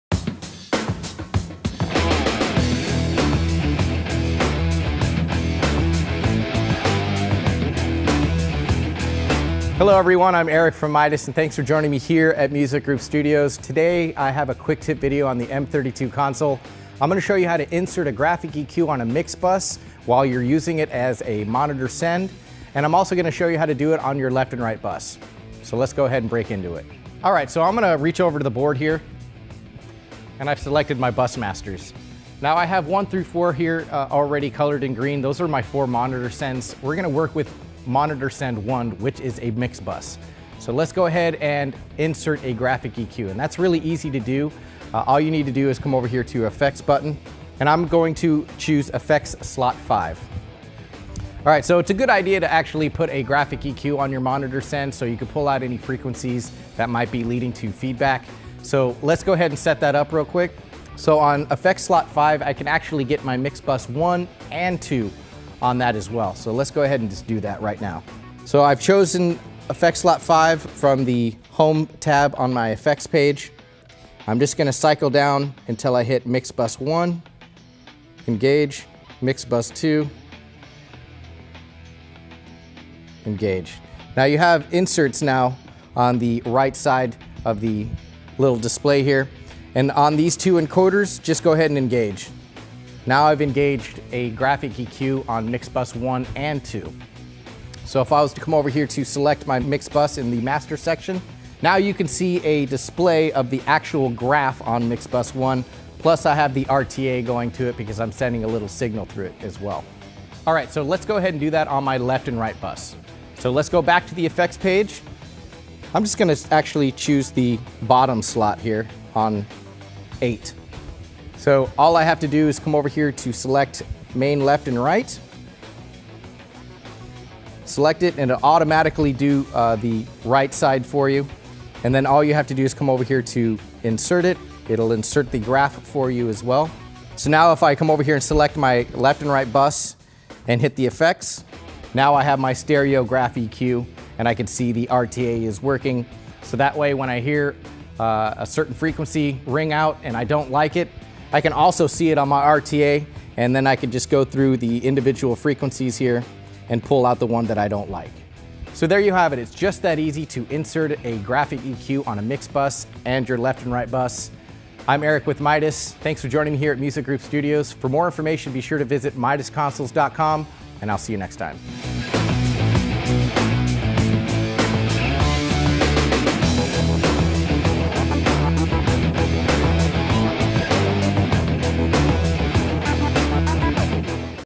MIDAS M32 Tutoriales en español: 14 Insertando un GEQ en el MixBus
En este video desarrollado por MIDAS, se muestra como insertar el Eq Gráfico interno en el bus de mezcla de la M32